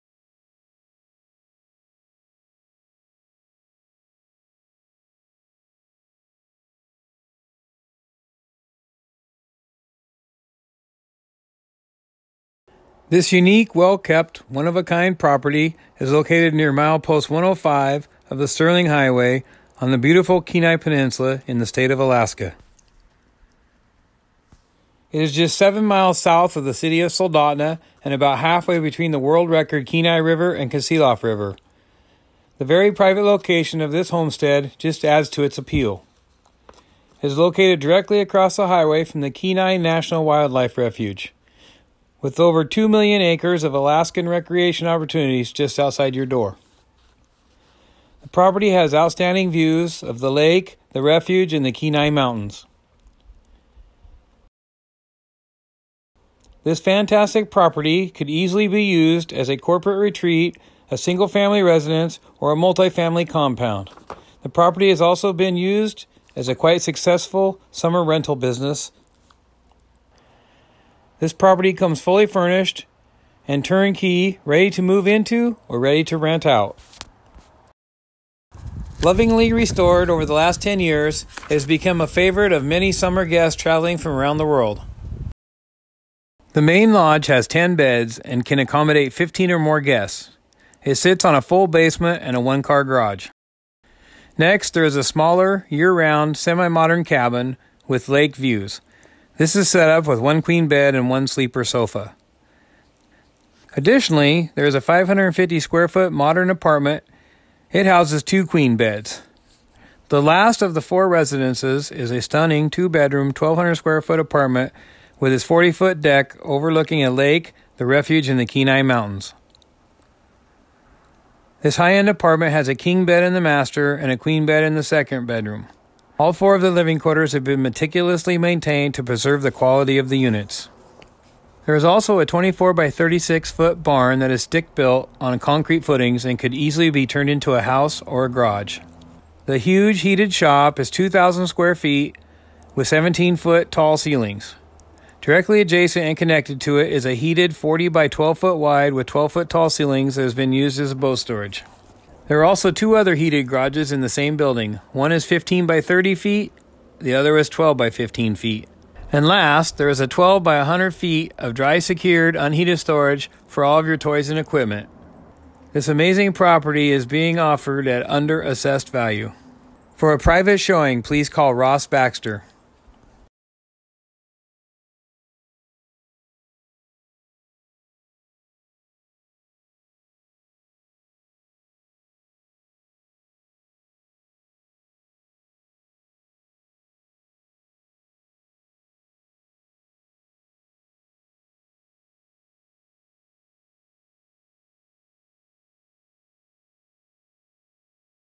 Exceptional Property for sale on the Kenai Peninsula. 3.4 acres huge shop awesome views 4 residences
One of a kind Family compound, corporate retreat or multi family rental on the Kenai Peninsula in Alaska. Gorgeous mountain and lake views

Huge shop on 3.4 acres private Soldotna.
4 residences. 24x36 barn. Lots of Airbnb income.

1200 square foot high-end condo.
3 bedroom 2 bath log lodge/house.
Nice 550 SF apartment.
Semi modern cabin.

Killer mountain and lake views. Across highway from Kenai National Wildlife Refuge. 7 miles from the Kenai River 3 miles from the Kasilof River.

Something For Everyone, LARGE VIEW PROPERTY! Includes a Log Home, Detached Cabin, HUGE 3500 S.F. Shop, (2) Apartments, a 864 S.F. Barn, 1,512 S.F. Wrap-around unheated Enclosed Storage,
3 other heated garage bays.
Landscaping, Many improvements, Rental Paradise or space for the entire family.
Has been successfully ran as summer rental business.